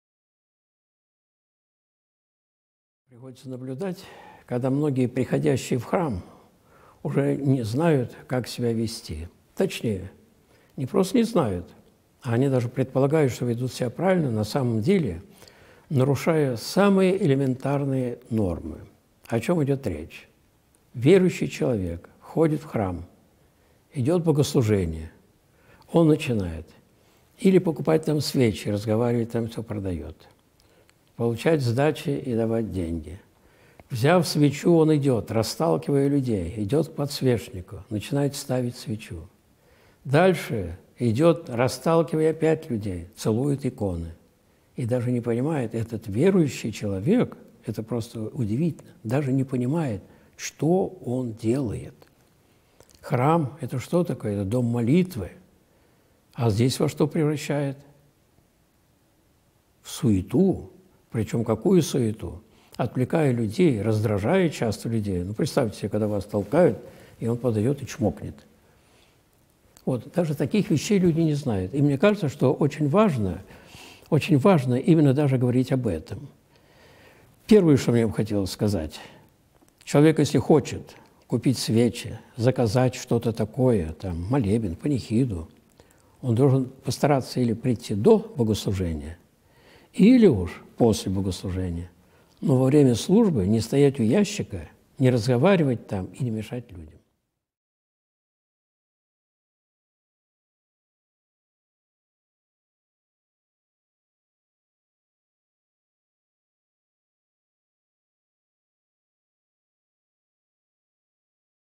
Как ставить свечи, заказывать молебны и требы в православном храме. А.И.Осипов
Приходится наблюдать, когда многие приходящие в храм уже не знают, как себя вести. Точнее, они даже предполагают, что ведут себя правильно, а на самом деле нарушают самые элементарные нормы. О чем идет речь? Верующий человек приходит в храм, идет богослужение, он начинает покупать свечи, разговаривать с тем, кто продает, получает деньги сдачи, взяв свечу он идет к подсвечнику, расталкивая людей, дальше опять идет толкаясь целовать иконы. Этот верующий человек, даже не понимает что он делает. Храм - это дом молитвы. А здесь во что превращает? В суету. Причем, в какую суету? Отвлекает людей, раздражает людей. Представьте себе, когда вас толкают, а потом он подходит и "чмокнет" икону.
Считаю, что очень важно об этом говорить. Первое, что мне хотелось бы сказать, что человек может купить свечи, заказать молебен или панихиду до богослужения или после. Но во время службы не стоять у ящика, не разговаривать там и не мешать другим.